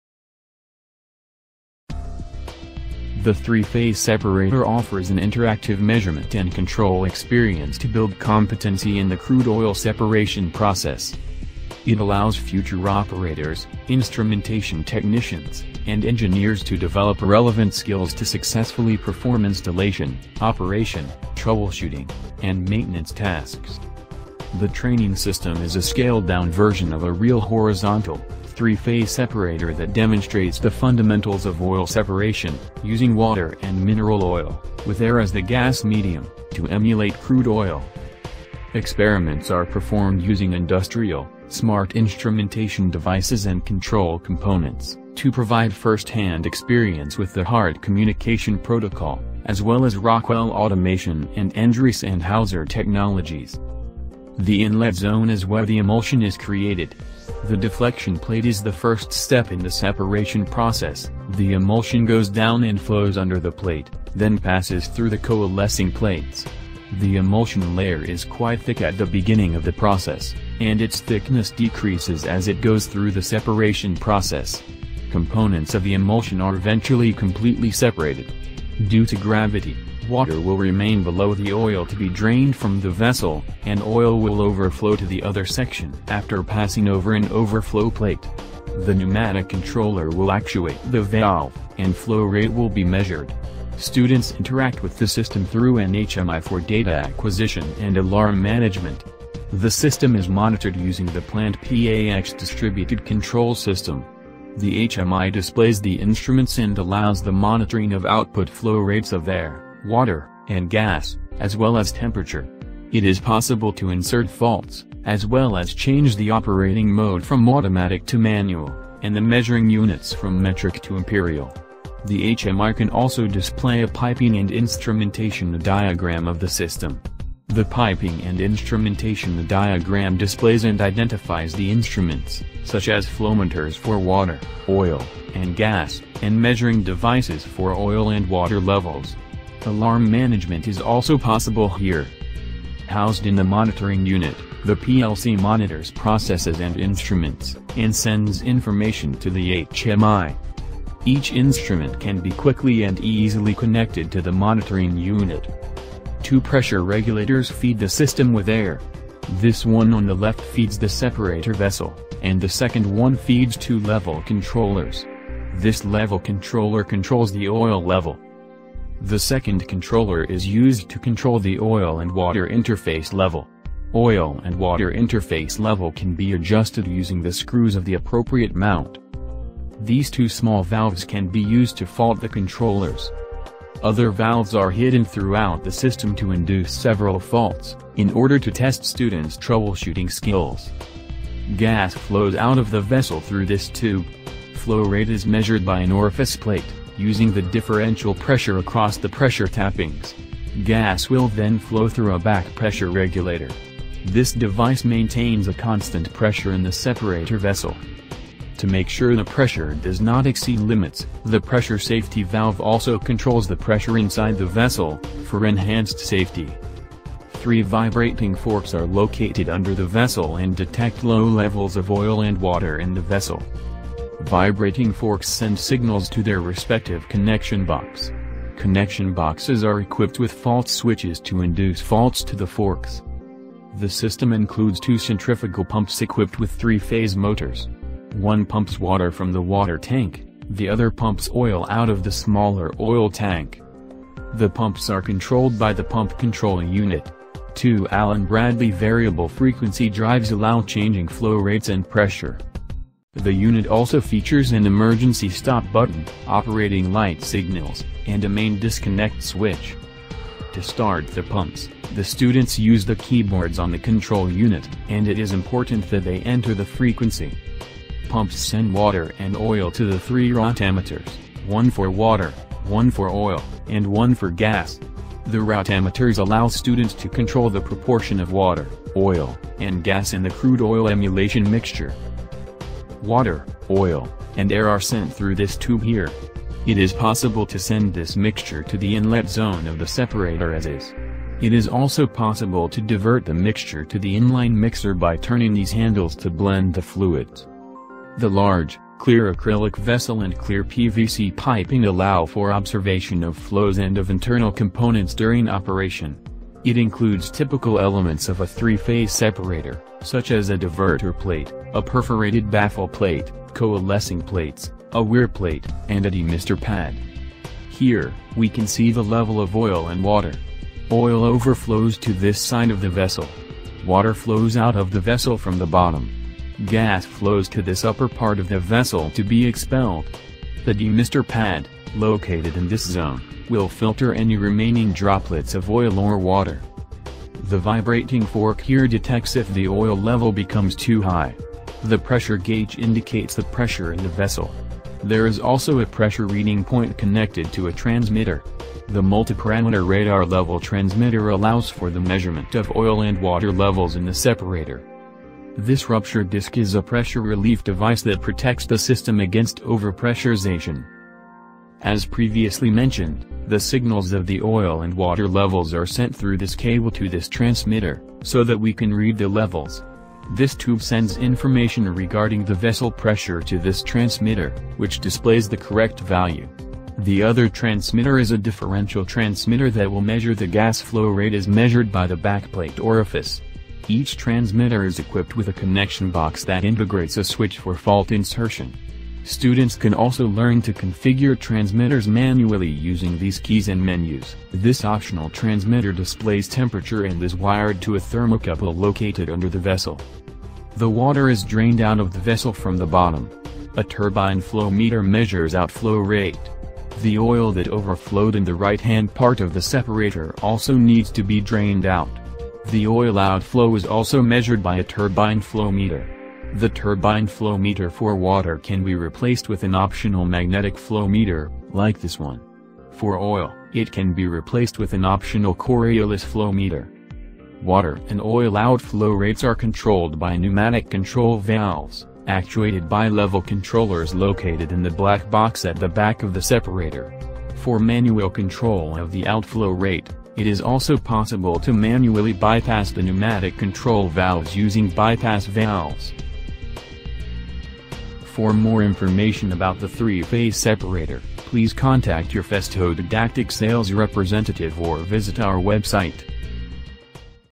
Three-Phase Separator: Main Components
Practical training in the crude oil separation process -- Working with industrial equipment, operators, instrumentation technicians, and engineers will develop relevant skills for successfully performing operation, troubleshooting, and maintenance tasks in the oil and gas industry.